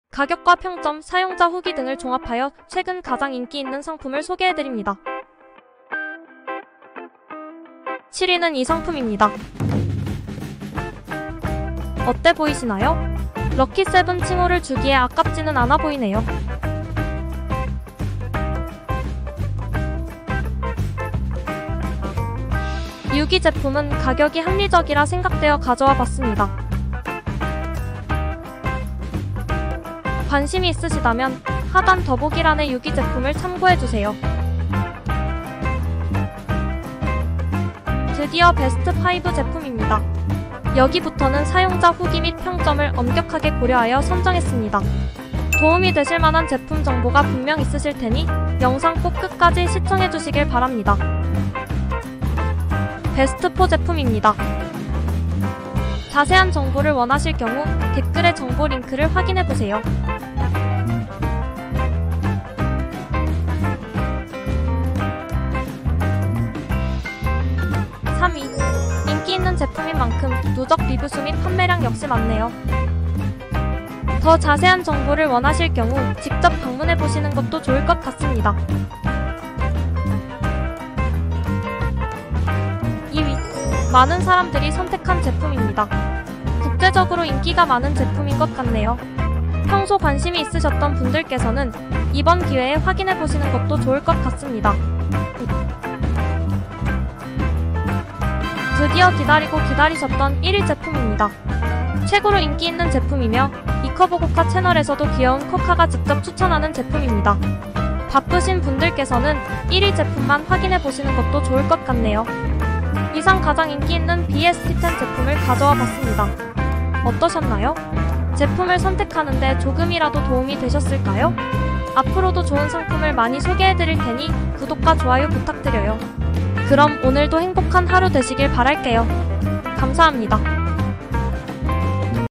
2023년 가장 핫한 럭키슈에뜨원피스 추천 BEST 7
** 정보 더보기 클릭! **
귀여운 쿼카가 정리한 럭키슈에뜨원피스 TOP 7를 정보를 전해드려요. // 소정의 수수료를 (주)쿠팡으로부터 지급받을 수 있으나, 구매 가격에는 영향을 미치지 않습니다.

[00:08] 7위 : [매장정품][럭키슈에뜨] 셔링반소매 원피스 LQDAM23500 LQDAW23500..

[00:23] 6위 : 프롬코 여성 튤립넥 긴팔 블랙 원피스 [자체제작 FRC77]..

[00:38] 5위 : 체크원피스 여름원피스..

[00:53] 4위 : [고품질 L-4XL] PINKFUL 원피스 여성 여름 무지 퍼프 오버핏 카라 브이..

[01:08] 3위 : 럭키슈에뜨 레이어드 롱원피스 LQDAW23600 나시원피스..

[01:23] 2위 : [매장정품][럭키슈에뜨] 셔링반소매 원피스 LQDAW23500..

[01:38] 1위 : 프롬코 우아한 여성 긴팔 빅플라워 롱 원피스 [자체제작 FRC106]..

꼭 저렴하게 구매하시길 응원합니다. 감사합니다.